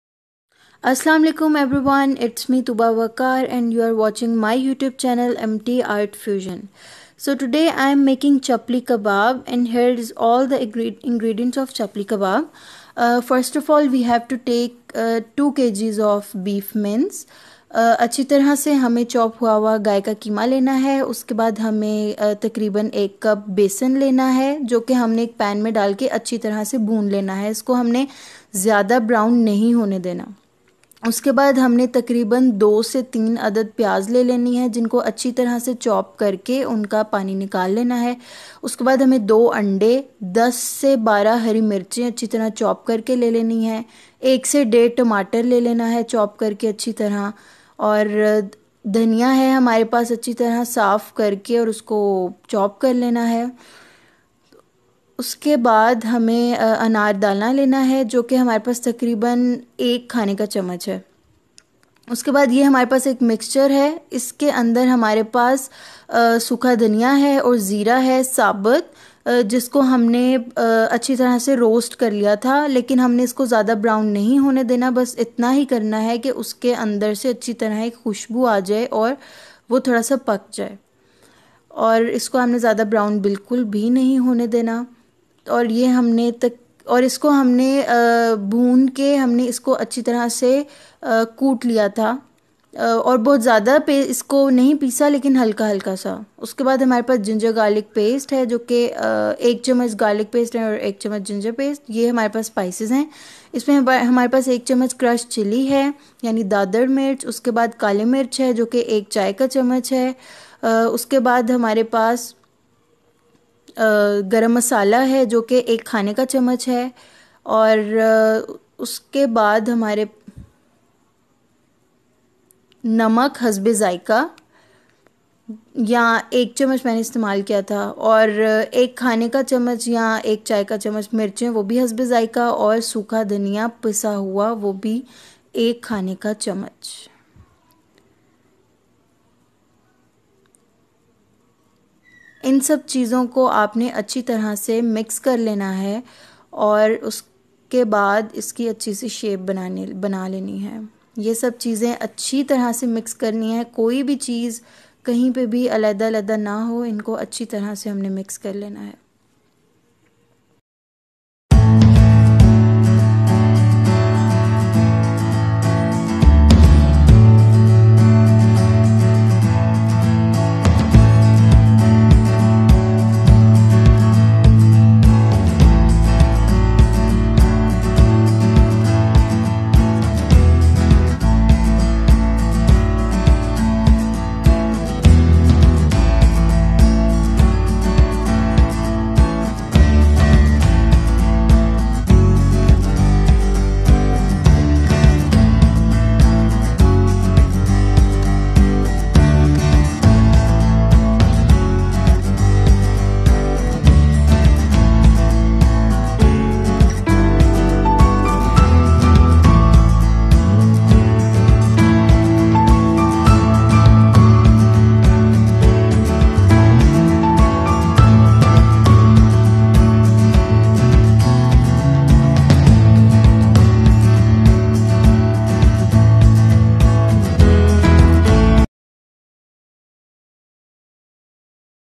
ChAPLI KEBAB RECIPE| PAKISTANI FOOD| MT Art Fusion
Its the recipe of full desi style chapli kebabs. Must try it
Its full of pakistani flavours.and spices.